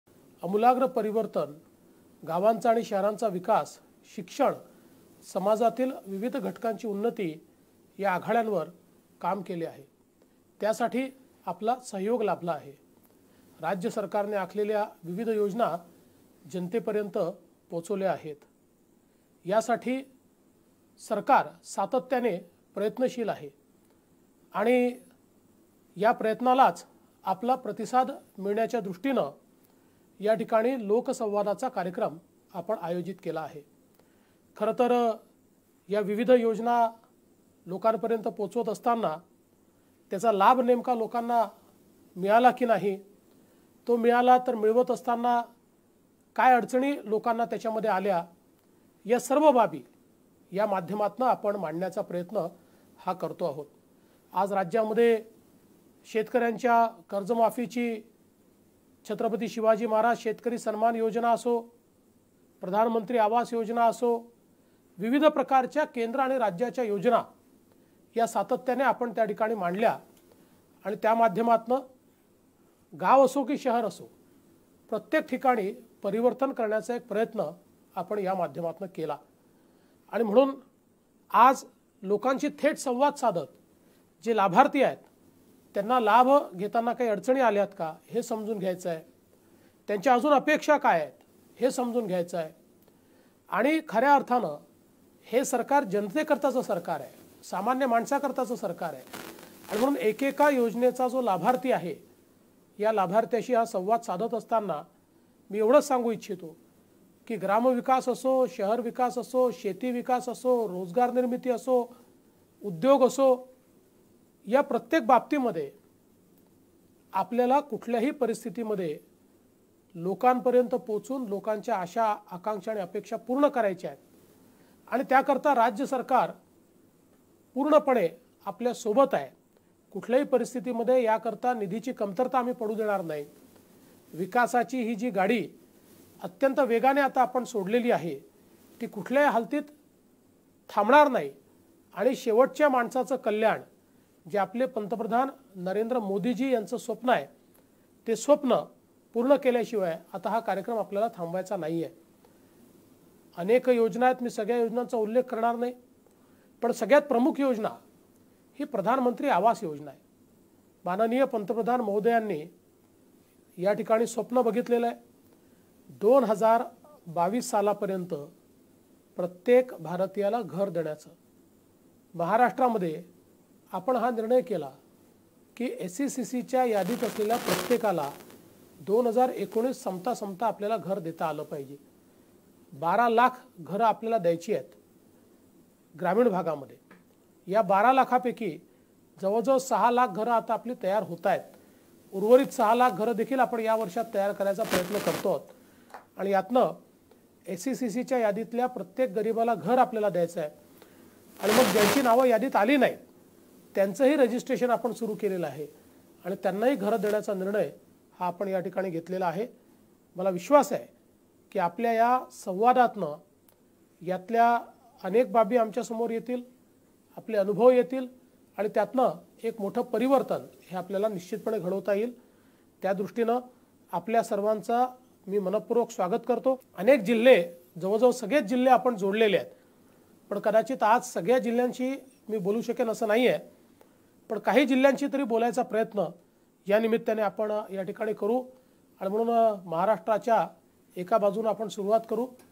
ग्रामीण भागात १२ लाख घरांचे नियोजन केले जात आहे.
ग्रामीण भागात १२ लाख घरांचे नियोजन केले जात आहे. ज्यातून एक मोठे परिवर्तन सर्वसामान्यांच्या जीवनात घडवता येईल. प्रधानमंत्री आवास योजनेच्या १३ विविध जिल्ह्यातील लाभार्थ्यांशी आज संवाद साधताना मला अतिशय आनंद झाला आणि अनेक उपयुक्त सूचनाही प्राप्त झाल्या.